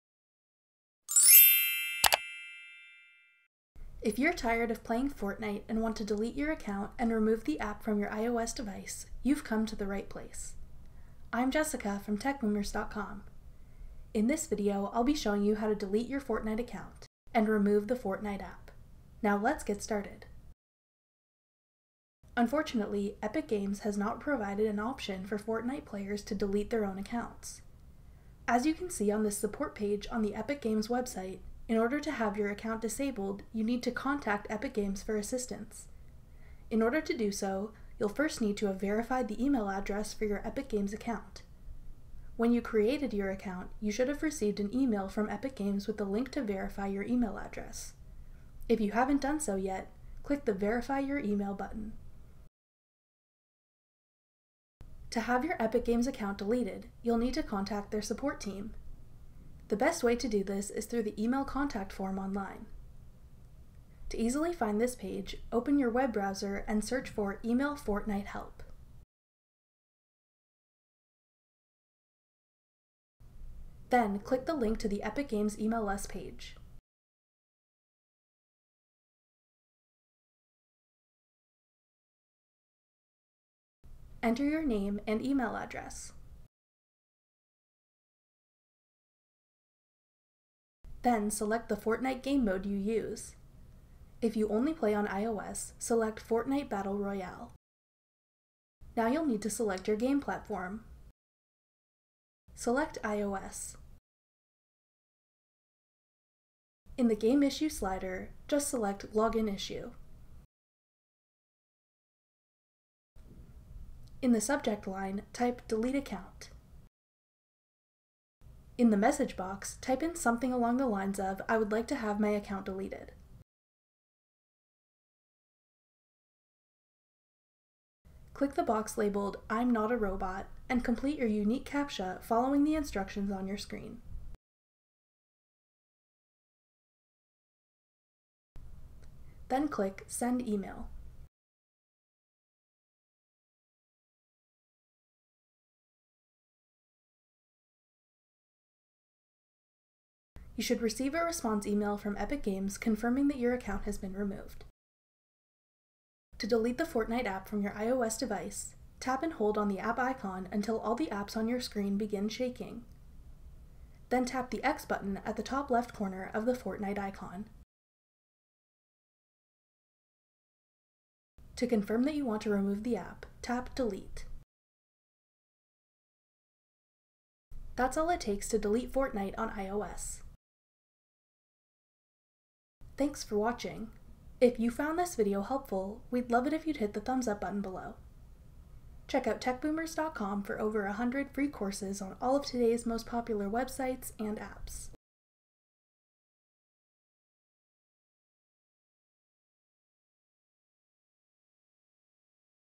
How to Delete Your Fortnite Account & App on iPhone or iPad
Learn how to delete your Fortnite account and remove the Fortnite app on your iOS devices like iPhone or iPad. We'll show you the process you need to complete to have your account deactivated, and how to delete the Fortnite app from your iOS device.

To delete your Fortnite account, you need to get in touch with their customer support team by filling out their online email contact form.

To delete the Fortnite app on iOS, tap and hold the app icon. Tap the X in the top left corner of the icon. To confirm, tap Delete.